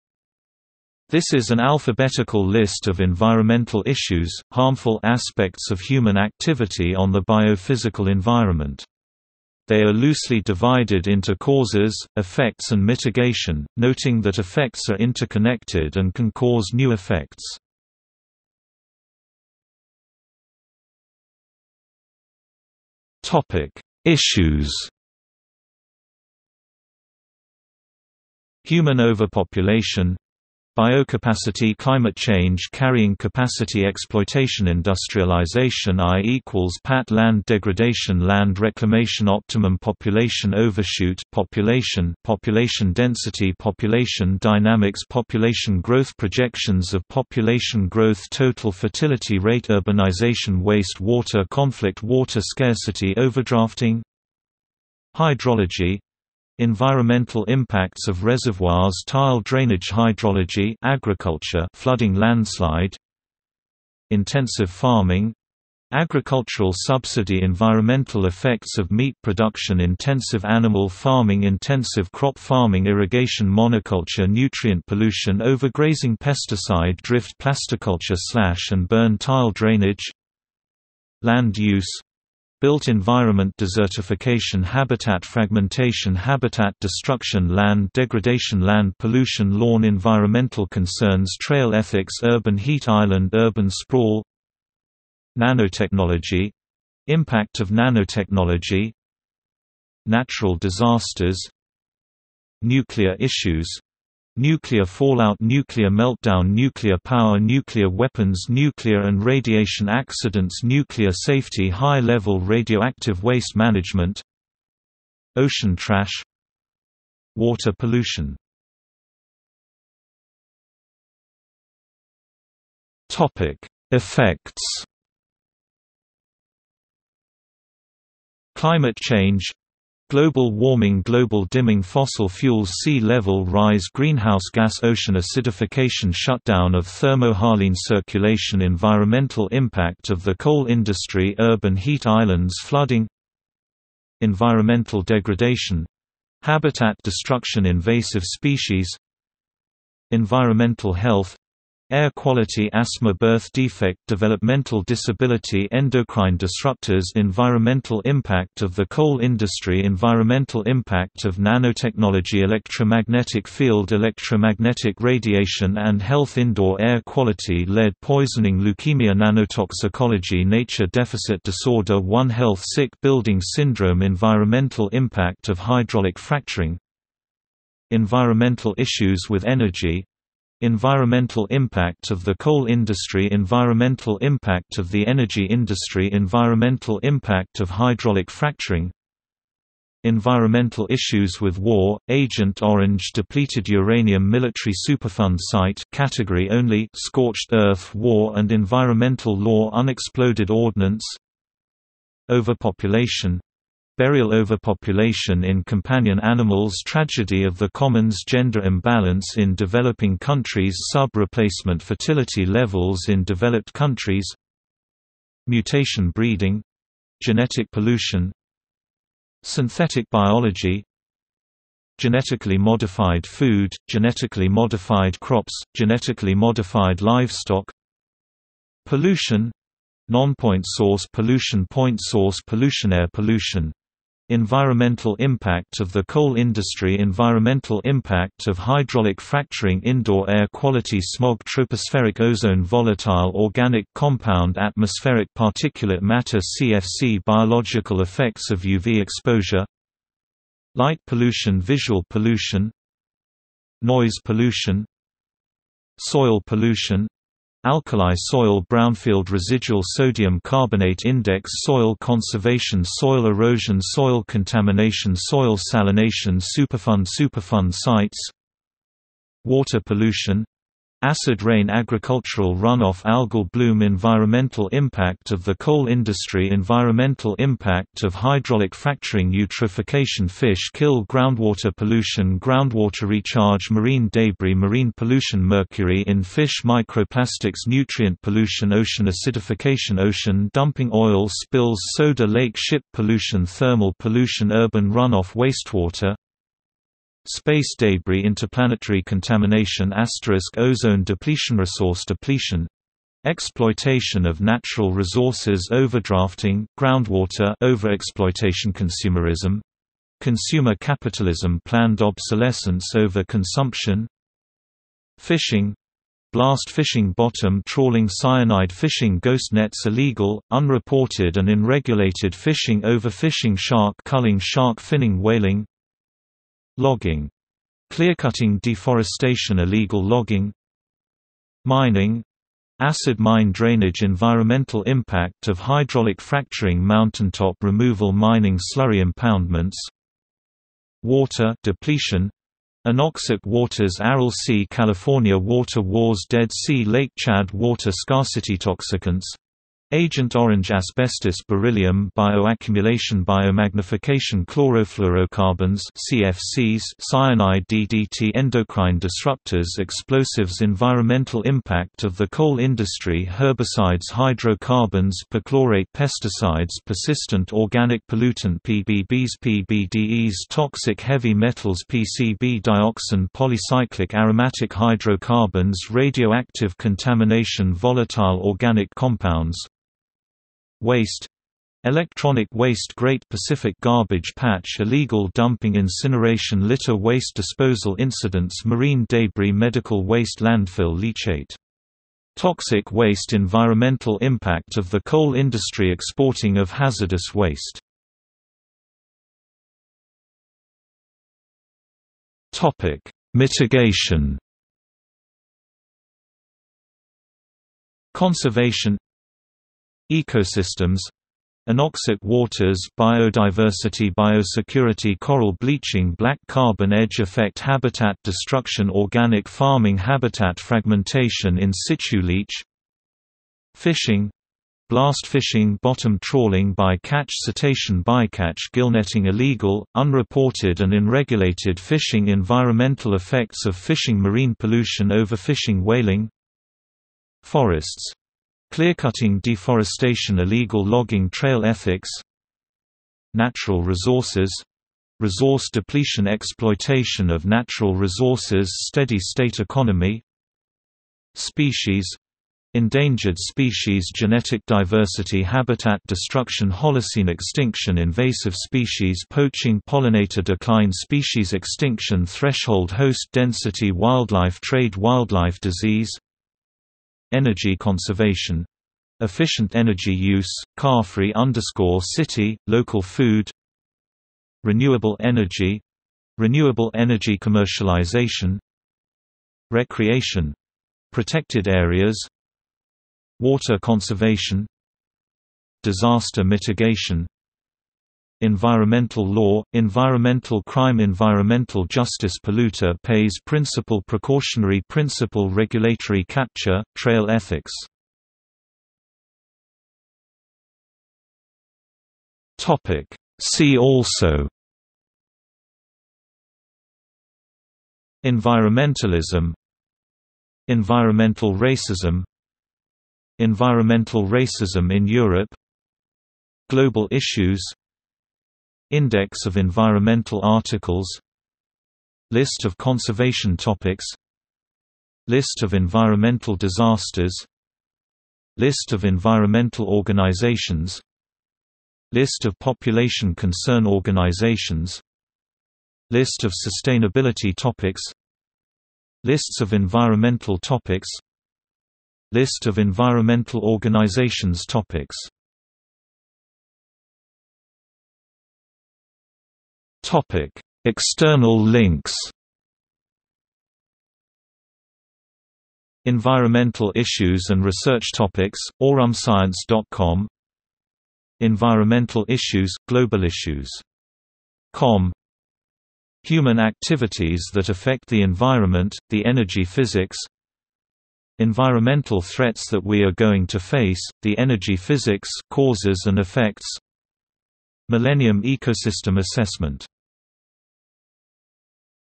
List of environmental issues | Wikipedia audio article
00:00:19 1 Issues
00:01:59 2 Effects
00:06:38 3 Mitigation
00:08:18 4 See also
00:09:06 5 External links

- Socrates

SUMMARY
This is an alphabetical list of environmental issues, harmful aspects of human activity on the biophysical environment. They are loosely divided into causes, effects and mitigation, noting that effects are interconnected and can cause new effects.